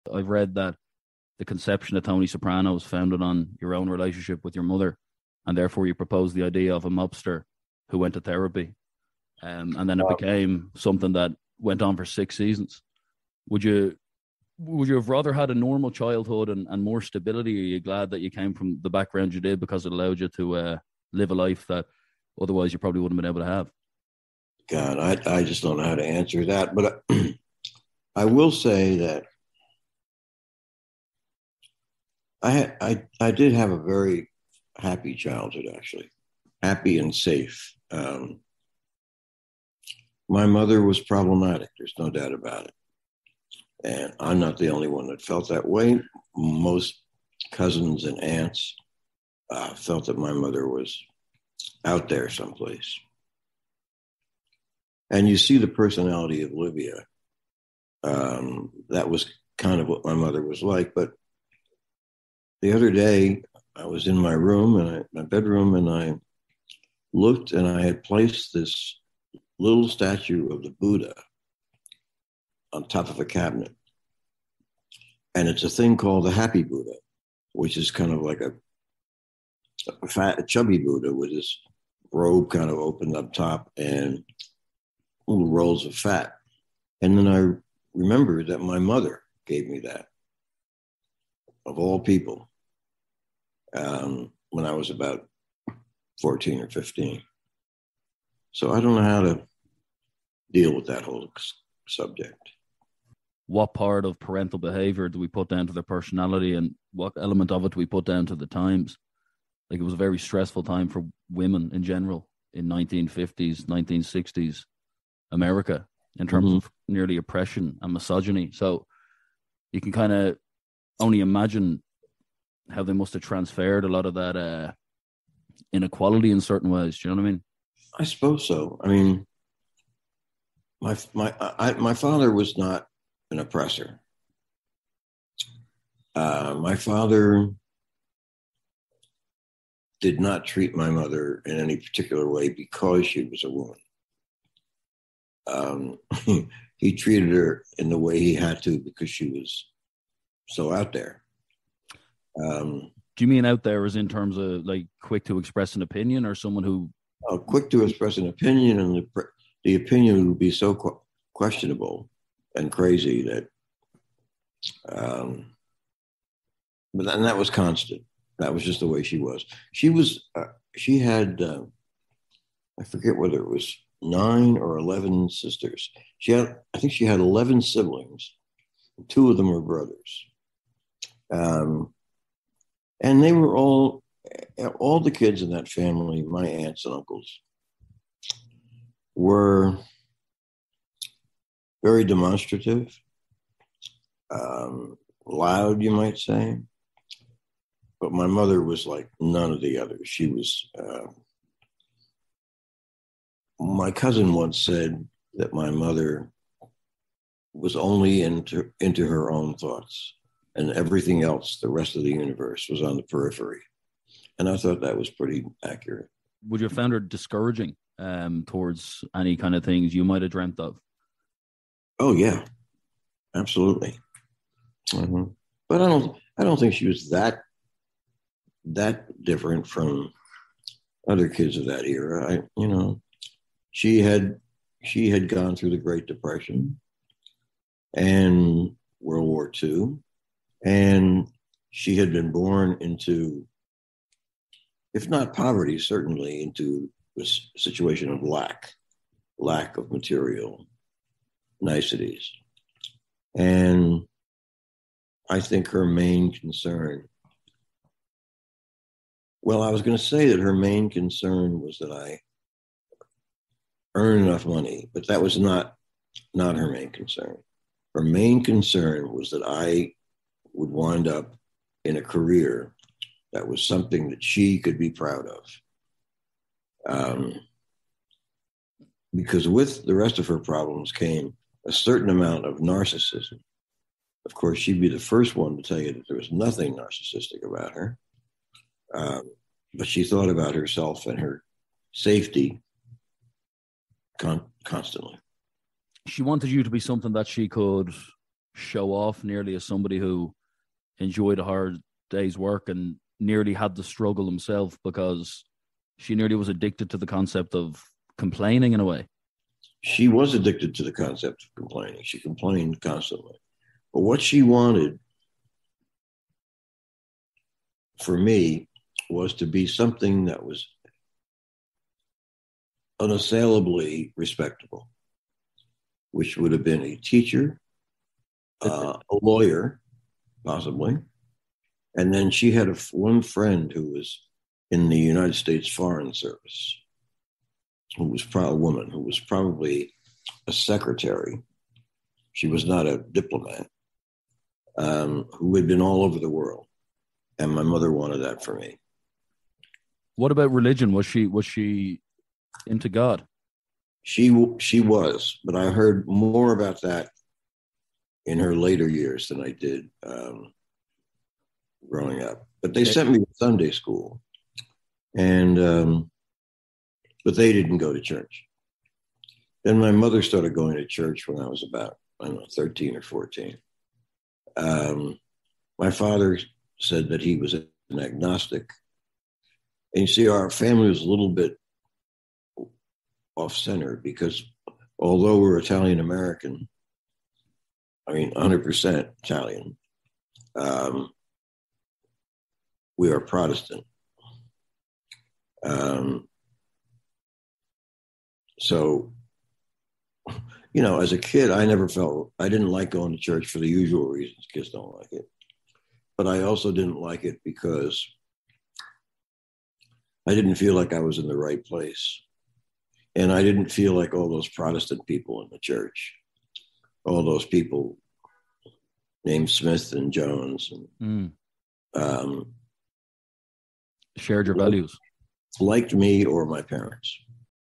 Liv Soprano WAS my real life mother - David Chase on creating The Sopranos
In this clip, Sopranos writer David Chase emotionally opens up on his relationship with his mother and how it inspired him to write the iconic TV classic, The Sopranos. David Chase delves into many topics throughout the podcast, which is a must listen for all interested in TV and film making.